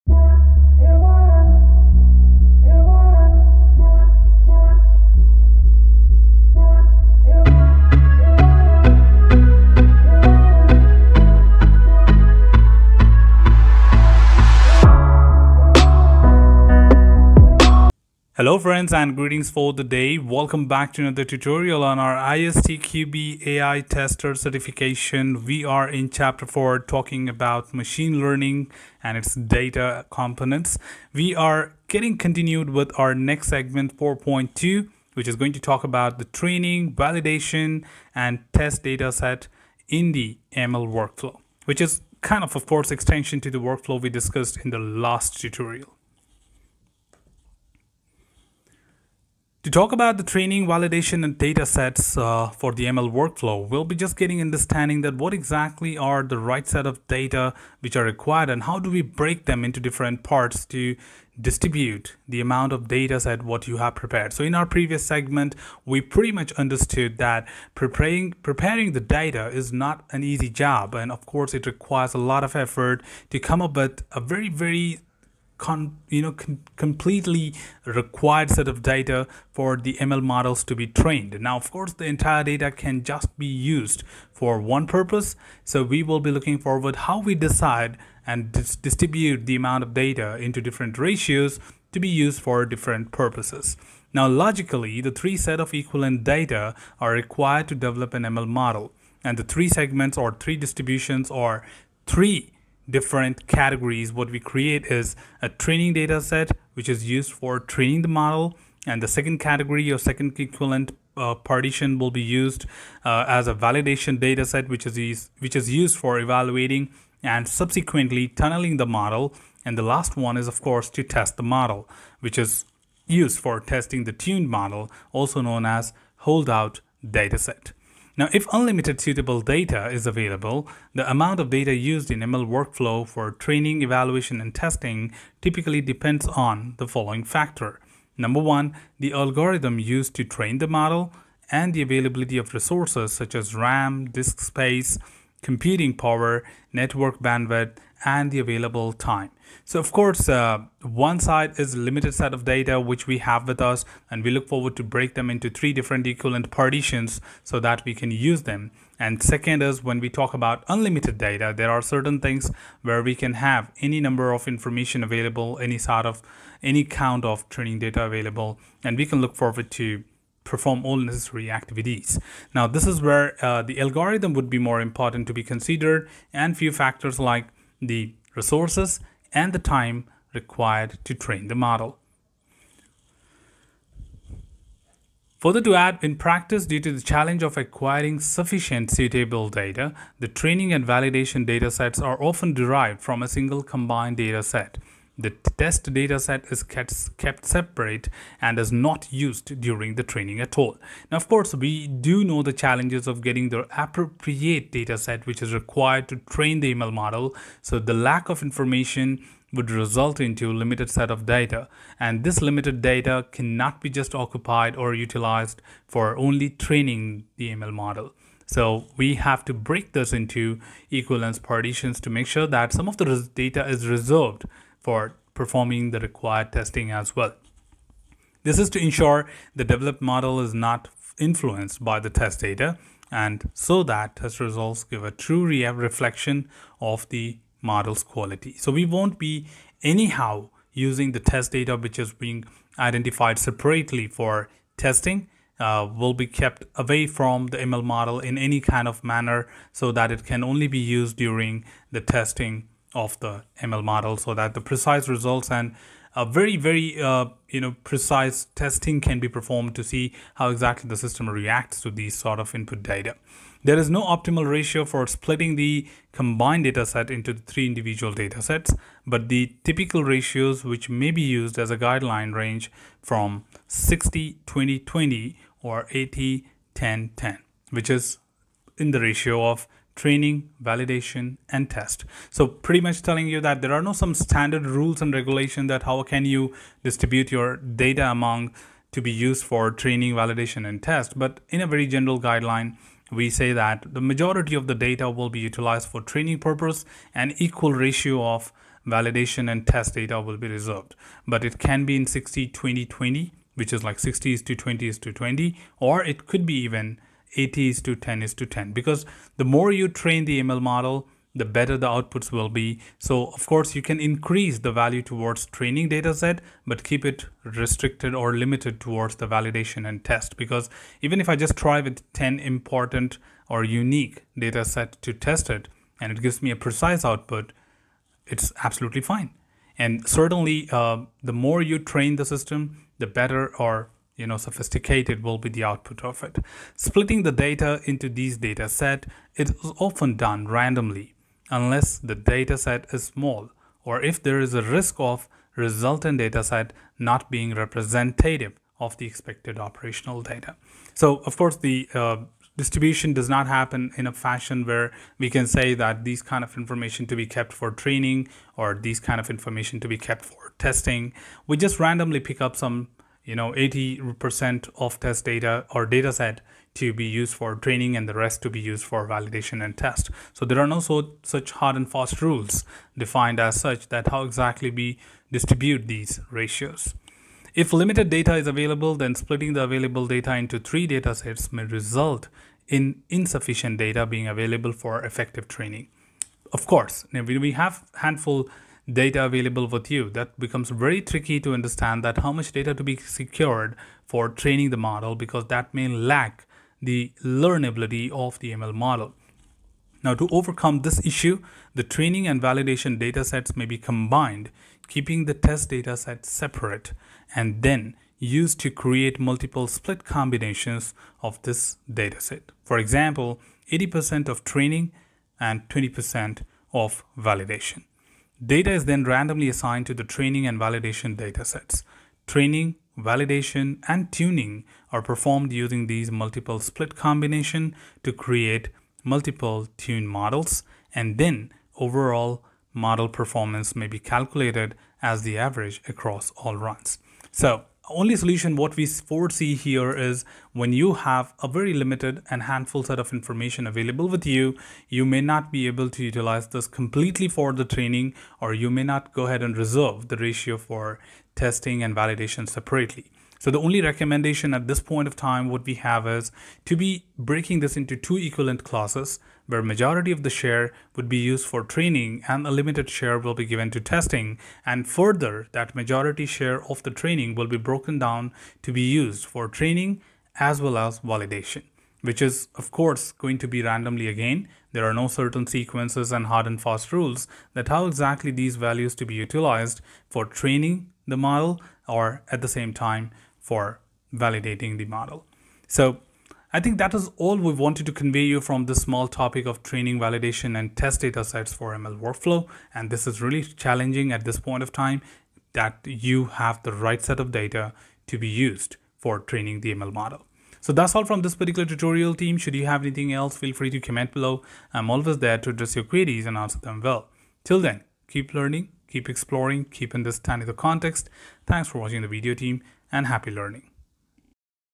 ISTQB AI Tester | Training, Validation and Test Data Set for ML Model | AI Tutorials
Hello Friends,

This tutorial will drive individuals about the Datasets for ML Model of AI about ISTQB AI Tester Certification of ISTQB Examination.

This series of tutorials will help individuals to prepare for the ISTQB Specialist AI Tester exam. This series consists of ISTQB Syllabus discussion, understanding, tips, tricks, and time management about the exam by Neeraj Kumar Singh.

AI Tester Certification,
Training Data for ML,
Validation Data for ML,
Testing Data for ML,